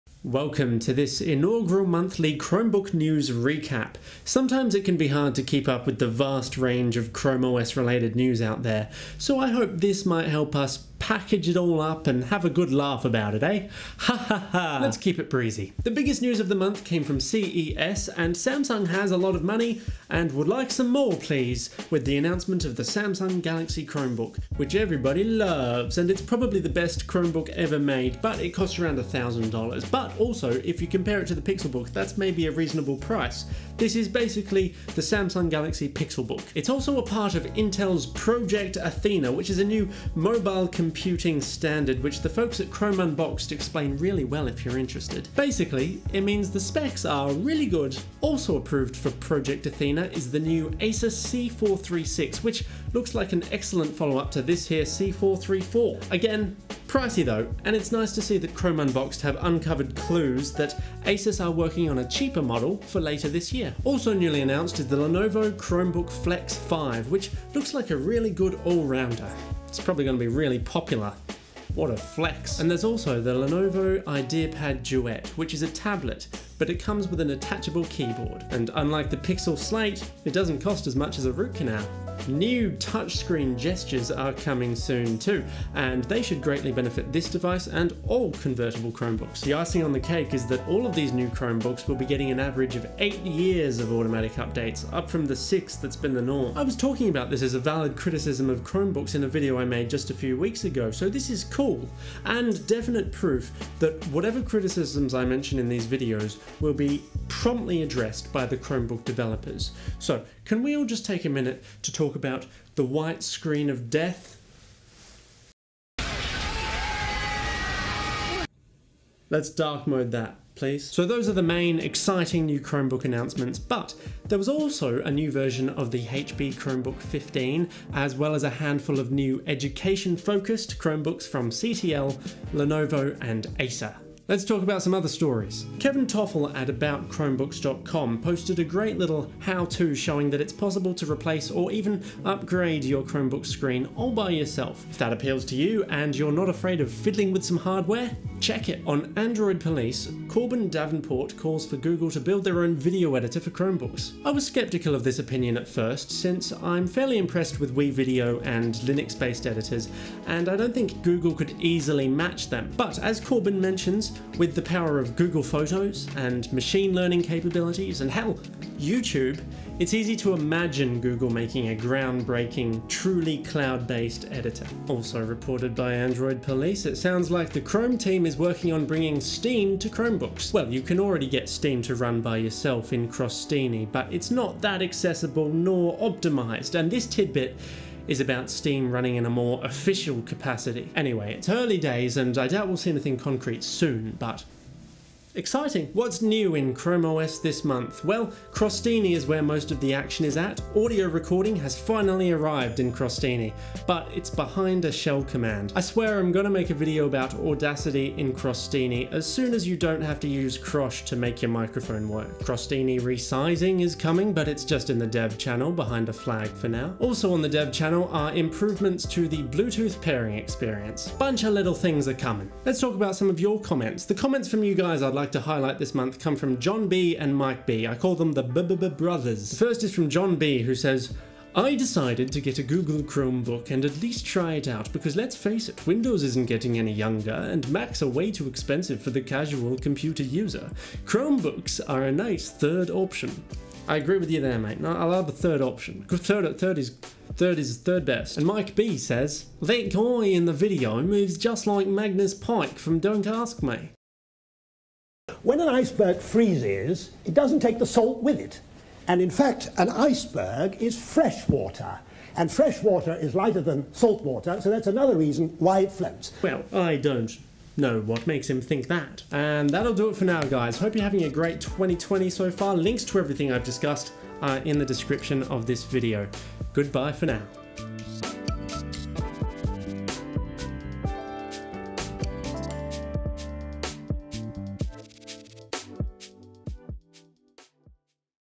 Chromebook News Roundup - January 2020
It was a busy month in Chrome world.

Asus likely to have budget versions of the C436 later in the year; Chrome Unboxed

Should Google build their own video editing platform?; Android Police

Sounds like Steam might come to Chromebooks officially; Android Police

How to get your microphone working in Crostini; About Chromebooks